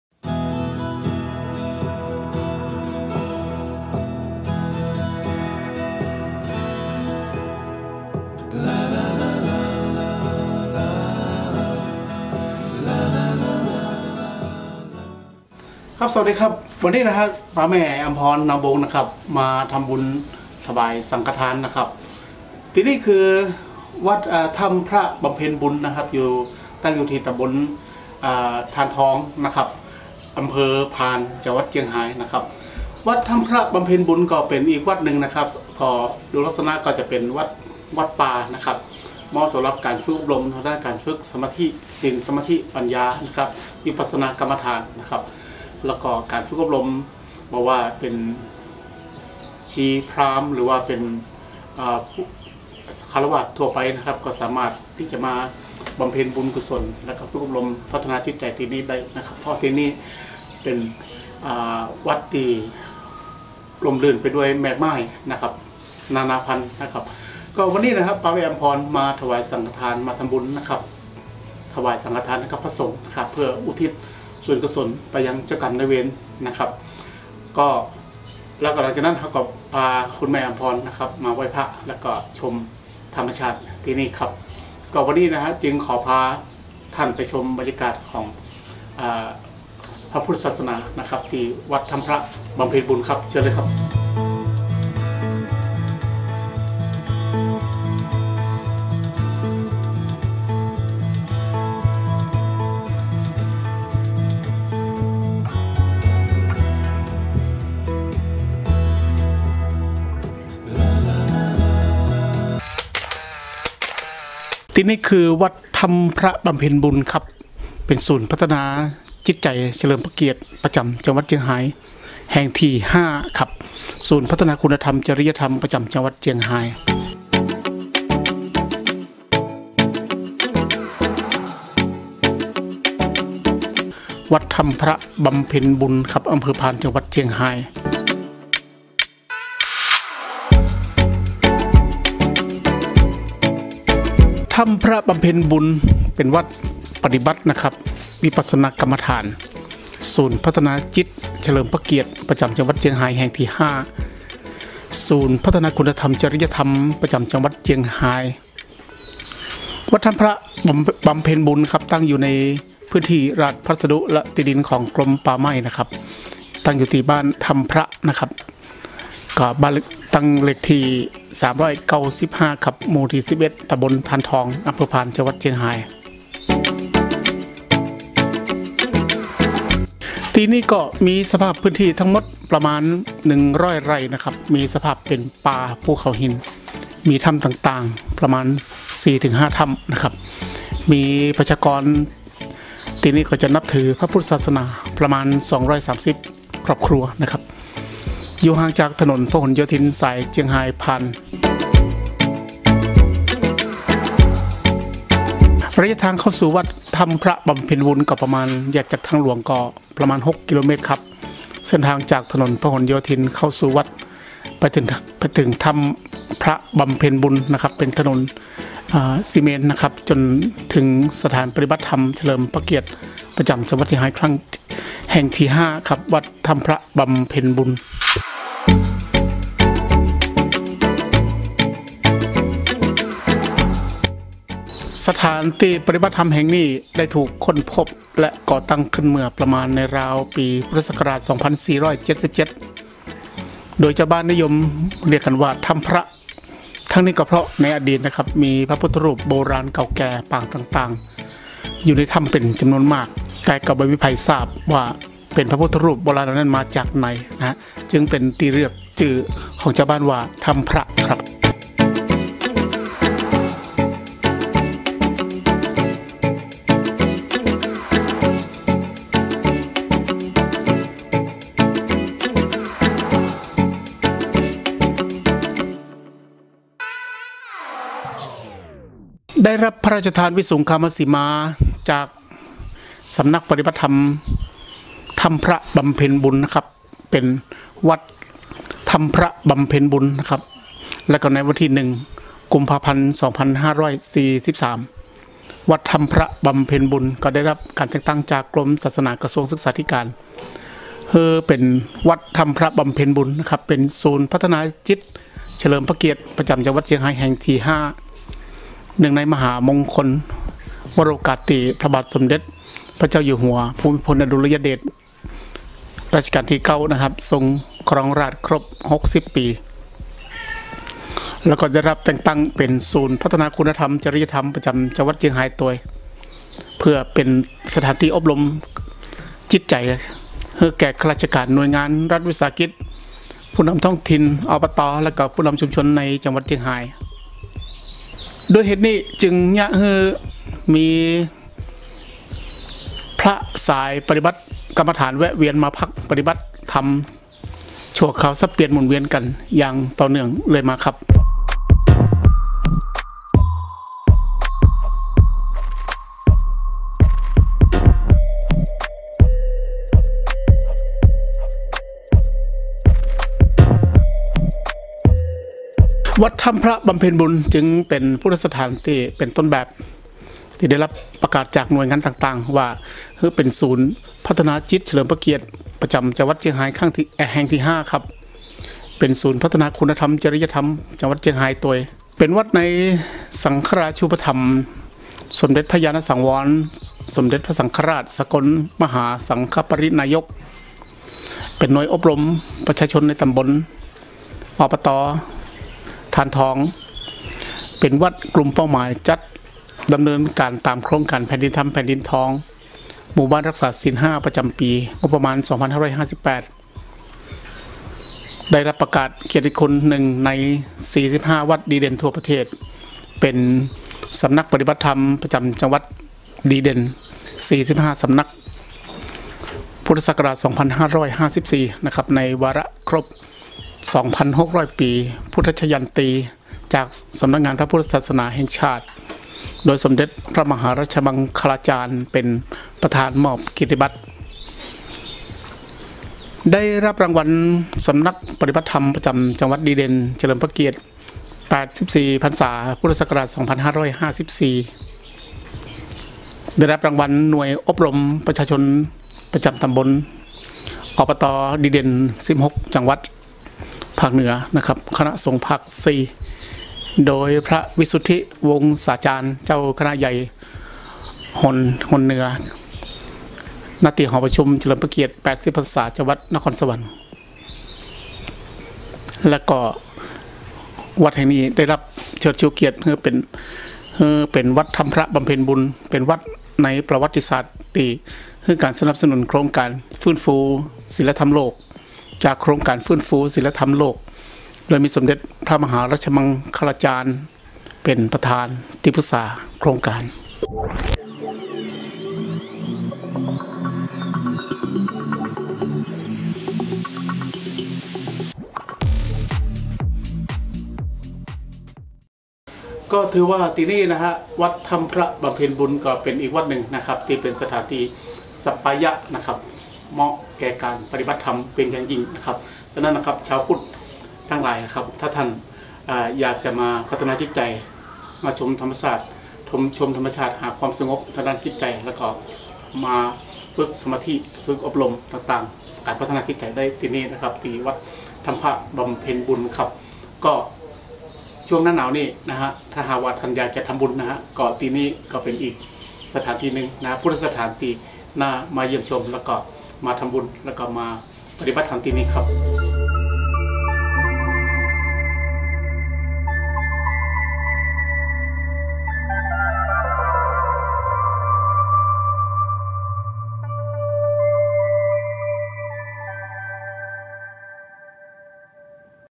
พาแม่ไหวพระที่วัดถ้ำพระบำเพ็ญบุญ
วันนี้..กิจกรรมการดูแลผู้สูงอายุที่บ้านครับ  เป็นกิจกรรมนอกสถานที่  พาแม่ไปไห้พระ และถวายสังฆทานแด่พระสงฆ์  ทำบุญอุทิศส่วนกุศลแด่เจ้ากรรมนายเวร ให้คุณแม่อำพร นามวงศ์ มีความสุข ความเจริญ อายุมั่นยืนยาว...

วั ด ถ้ำ พ ร ะ บำ เ พ็ ญ บุ ญ ศูนย์พัฒนาจิตเฉลิมพระเกียรติประจำจังหวัดเชียงรายแห่งที่ ๕ ศูนย์พัฒนาคุณธรรม-จริยธรรม ประจำจังหวัดเชียงราย
วัดถ้ำพระบำเพ็ญบุญ อ.พาน จ.เชียงราย... วัดถ้ำพระบำเพ็ญบุญ เป็นวัดปฏิบัติวิปัสสนากรรมฐาน ศูนย์พัฒนาจิตเฉลิมพระเกียรติประจำจังหวัดเชียงราย แห่งที่ ๕ ศูนย์พัฒนาคุณธรรม-จริยธรรม ประจำจังหวัดเชียงราย

วัดถ้ำพระบำเพ็ญบุญ ตั้งอยู่ในพื้นที่ราชพัสดุและที่ดินของกรมป่าไม้บ้านถ้ำพระ (ห้วยหลวง-บ้านนิคมแม่ลาว) เลขที่ ๓๙๕ หมู่ ๑๑ ตำบลธารทอง อำเภอพาน จังหวัดเชียงราย ๕๗๒๕๐ โทรศัพท์-โทรสาร (๐๕๓) ๑๘๔ ๓๒๕-๖

สภาพพื้นที่ทั้งหมดประมาณ ๑๐๐ ไร่ มีสภาพเป็นป่าภูเขาหิน มีถ้ำต่างๆ อีกประมาณ ๔-๕ ถ้ำ มีประชากรนับถือพระพุทธศาสนาประมาณ ๒๓๐ ครอบครัว อยู่ห่างจากถนนพหลโยธินสายเชียงราย-พาน กม.๘๐๒-๘๐๓ ระยะทางเข้าสู่วัดถ้ำพระบำเพ็ญบุญประมาณ ๖ กิโลเมตร เส้นทางจากถนนพหลยโยธินเข้าสู่วัดถ้ำพระบำเพ็ญบุญ เป็นถนนซีเมนต์จนถึงสถานปฏิบัติธรรมเฉลิมพระเกียรติประจำจังหวัดเชียงรายแห่งที่ ๕
วัดถ้ำพระบำเพ็ญบุญ สถานที่ปฏิบัติธรรมแห่งนี้ได้ถูกค้นพบและก่อตั้งขึ้นเมื่อประมาณในราวปีพุทธศักราช ๒๔๗๗ โดยชาวบ้านนิยมเรียกกันว่า “ถ้ำพระ” ทั้งนี้เพราะในอดีตมีพระพุทธรูปโบราณเก่าแก่ปางต่างๆ อยู่ในถ้ำเป็นจำนวนมากโดยก็มิมีใครทราบว่าพระพุทธรูปโบราณเหล่านี้มาจากไหน จึงเป็นที่เรียกกันของชาวบ้านว่า “ถ้ำพระ" ได้รับพระราชทานวิสุงคามสีมาจากสำนักปฏิบัติธรรม ถ้ำพระบำเพ็ญบุญ เป็น วัดถ้ำพระบำเพ็ญบุญ และในวันที่ ๑ กุมภาพันธ์ ๒๕๔๓ วัดถ้ำพระบำเพ็ญบุญ ได้รับการแต่งตั้งจาก กรมศาสนา กระทรวงศึกษาธิการ ให้วัดถ้ำพระบำเพ็ญบุญ เป็น ศูนย์พัฒนาจิตเฉลิมพระเกียรติประจำจังหวัดเชียงราย แห่งที่ ๕ เนื่องในมหามงคลวโรกาสที่ พระบาทสมเด็จพระเจ้าอยู่หัวภูมิพลอดุลยเดชมหาราช ทรงครองราชย์ครบ ๖๐ ปี และได้รับแต่งตั้งเป็น "ศูนย์พัฒนาคุณธรรม-จริยธรรม ประจำจังหวัดเชียงราย" เพื่อเป็นสถานที่อบรมข้าราชการ หน่วยงาน รัฐวิสาหกิจ อบรมผู้นำ อ.บ.ต. และผู้นำชุมชนในจังหวัดเชียงราย ด้วยเหตุนี้จึงทำให้มีพระสายปฏิบัติกรรมฐานแวะเวียนมาพักปฏิบัติธรรมชั่วคราว สับเปลี่ยนหมุนเวียนอยู่อย่างนี้เรื่อยมา.
วัดถ้ำพระบำเพ็ญบุญ ได้รับแต่งตั้งจากหน่ายงานต่างๆ • เป็นศูนย์พัฒนาจิตเฉลิมพระเกียรติ ประจำจังหวัดเชียงราย แห่งที่ ๕ • เป็นศูนย์พัฒนาคุณธรรม-จริยธรรม ประจำจังหวัดเชียงราย • เป็นวัดในสังฆราชูปถัมภ์ สมเด็จพระญาณสังวร สมเด็จพระสังฆราช สกลมหาสังฆปรินายก • เป็นหน่วยอบรมประชาชนประจำตำบล อ.บ.ต.ธารทอง • เป็นวัดกลุ่มเป้าหมายจัดดำเนินการตามโครงการแผ่นดินธรรม แผ่นดินทอง หมู่บ้านรักษาศีล ๕ ประจำปีงบประมาณ ๒๕๕๘ • ได้รับการประกาศเกียรติคุณ ๑ ใน ๔๕ วัดดีเด่นทั่วประเทศ เป็น "สำนักปฏิบัติธรรมประจำจังหวัดดีเด่น ๔๕ สำนัก" พุทธศักราช ๒๕๕๔ในวาระครบ ๒๖๐๐ ปีพุทธชยันตี จากสำนักงานพระพุทธศาสนาแห่งชาติ โดยสมเด็จพระมหารัชมังคลาจารย์ เป็นประธานมอบเกียรติบัตร • ได้รับรางวัลสำนักปฏิบัติธรรมประจำจังหวัดดีเด่น เฉลิมพระเกียรติ ๘๔ พรรษา พุทธศักราช ๒๕๕๔ • ได้รับรางวัลหน่วยอบรมประชาชนประจำตำบล(อ.ป.ต.)ดีเด่น ๑๖ จังหวัดหนเหนือคณะสงฆ์ภาค ๔ โดยพระวิสุทธิวงศาจารย์ เจ้าคณะใหญ่หนเหนือ ณ หอประชุมเฉลิมพระเกียรติ ๘๐ พรรษา จ.นครสวรรค์ • ได้รับเชิดชูเกียรติให้วัดถ้ำพระบำเพ็ญบุญ เป็นวัดในประวัติศาสตร์ ที่ให้การสนับสนุนโครงการฟื้นฟูศีลธรรมโลก จากโครงการฟื้นฟูศีลธรรมโลกโดยมีสมเด็จพระมหารัชมังคลาจารย์เป็นประธานที่ปรึกษาโครงการ...